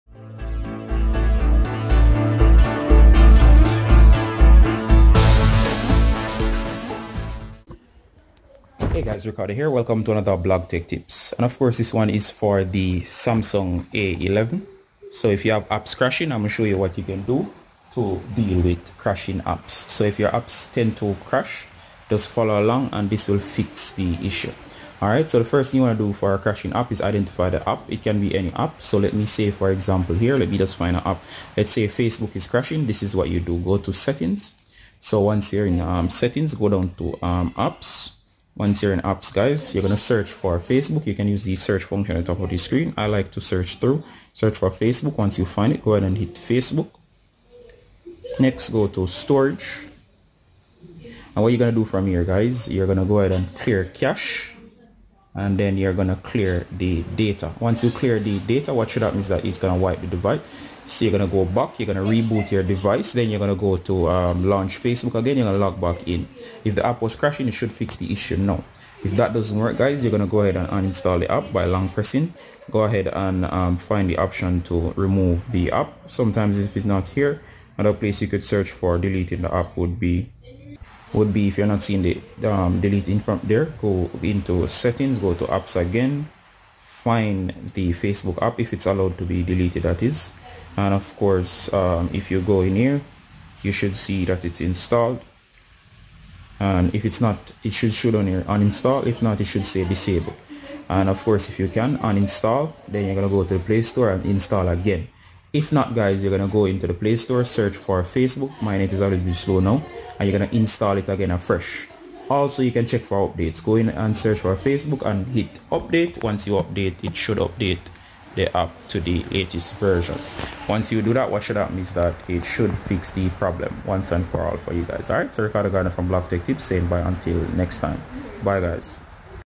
Samsung Galaxy A11 Apps Crashing Fix
This is How to Fix Crashing Apps on the Samsung Galaxy A11. If your Apps are crashing or closing randomly with any Apps do the following to fix the problem and have Apps working again.